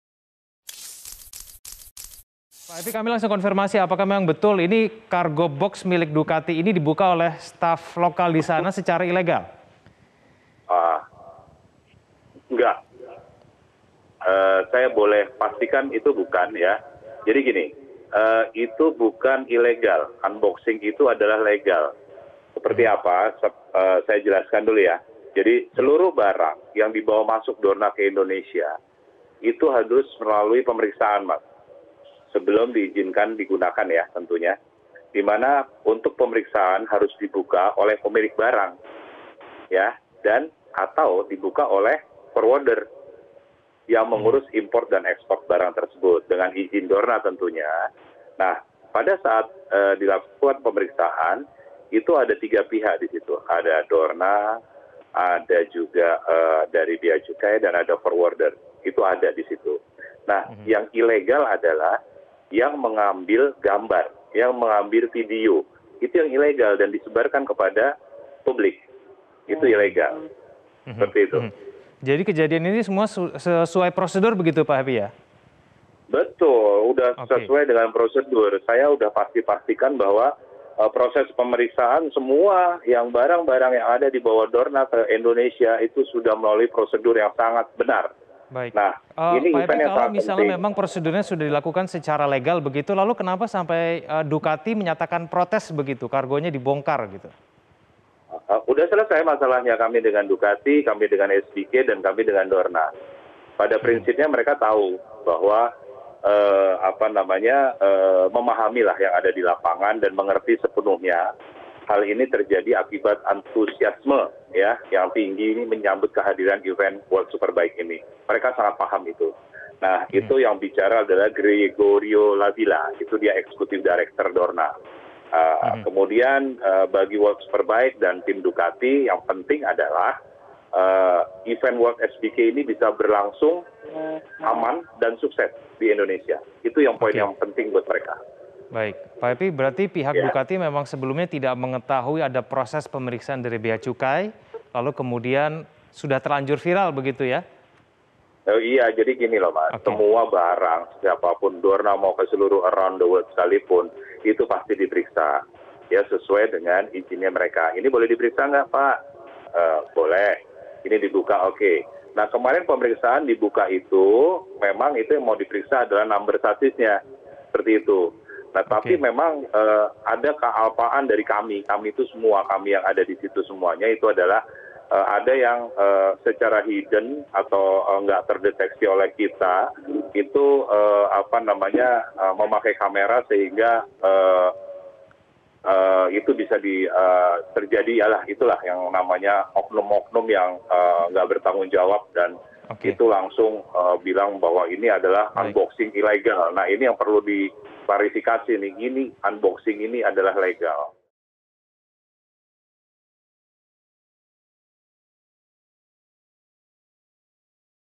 MGPA Bantah Pembukaan Boks Kargo Ducati Ilegal
Viral panitia dari Mandalika Grand Prix Association (MGPA) diduga membuka secara ilegal boks kargo Ducati dan mengutak-atik motor pembalap. Hal ini berawal dari koresponden media asal Swiss yang membagikan video dan foto-foto diduga panitia yang membuka boks kargo Ducati milik Michael Rinaldi.

Namun, hal ini dibantah oleh Direktur Strategi dan Komunikasi MGPA Happy K Harianto. Menurutnya pembukaan boks itu tidak ilegal, yang ilegal adalah pengambilan gambar.